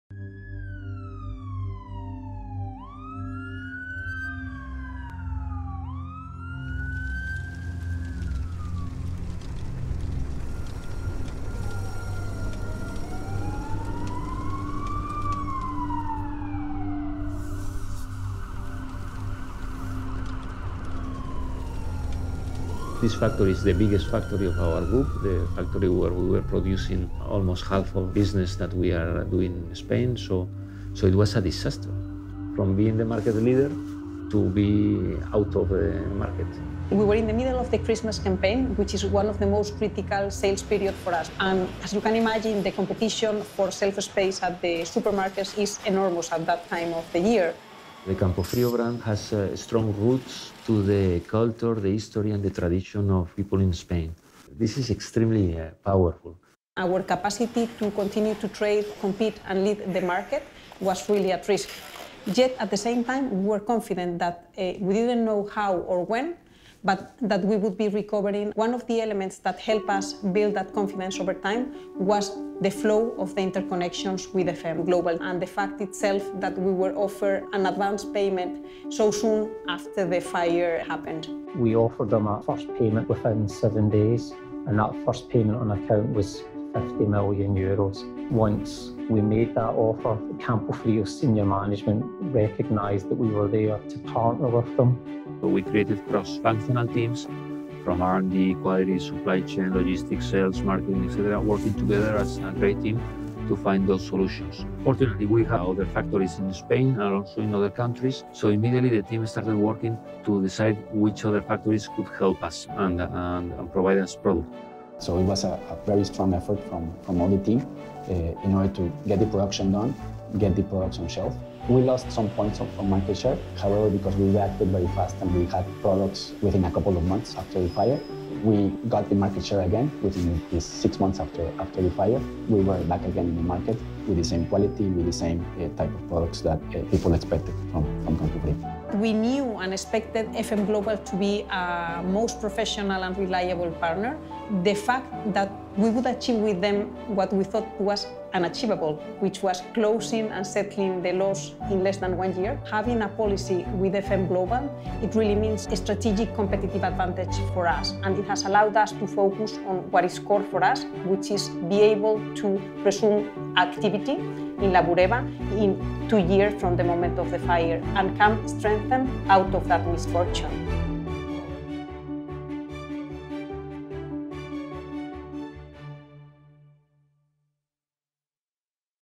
Campofrio: A Story of Recovery and Resilience
The Madrid-based Campofrio Food Group, one of Europe's leading purveyors of processed meat with net sales of 2 billion euros, suffered a catastrophic fire in late 2014 at their biggest and most important facility.

This video describes the fire that destroyed the Campofrio Food Group’s Burgos, Spain, facility and how the company collaborated with FM Global to construct a new highly protected plant in its place.